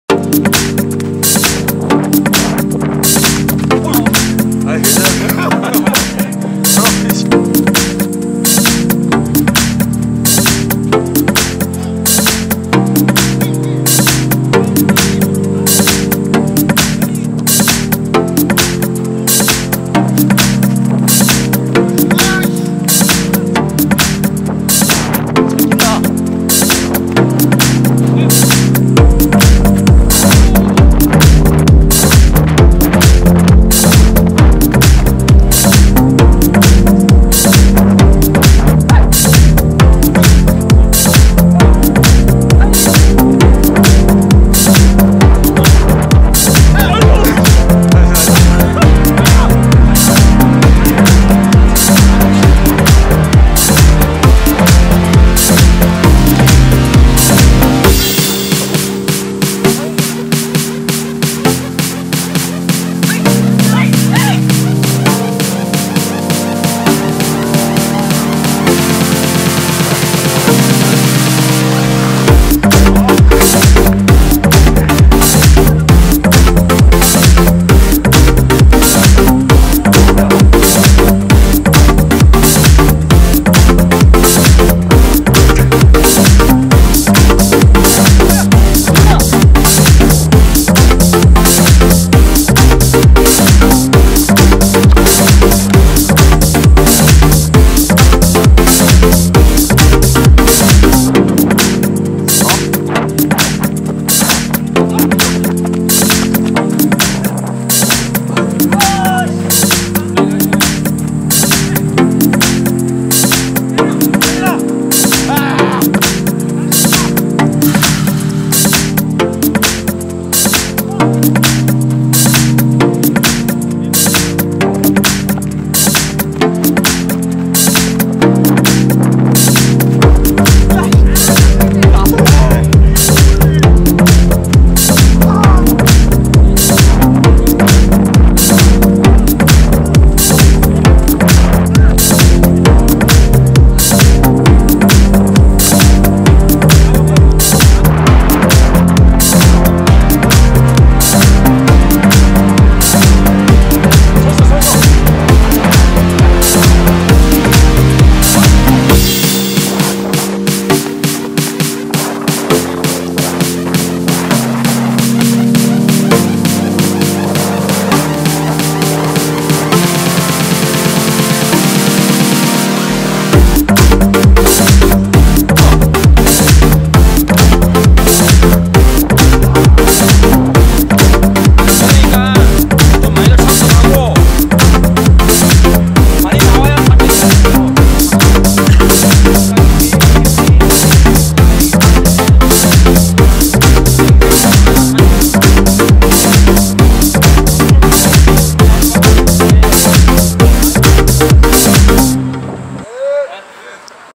[소니액션캠] sony actioncam FDR-X3000 - 축구 soccer
소니 액션캠   fdr-x3000 의 손떨림 보정(B.O.S.S) 덕분에
이전 제품보다 좀더 안정적인 영상을 남길수 있게되었습니다.
촬영은 가슴마운트와 손으로 들고 촬영했으며
XAVX S 1080 60P 100mbps 로 저장했습니다.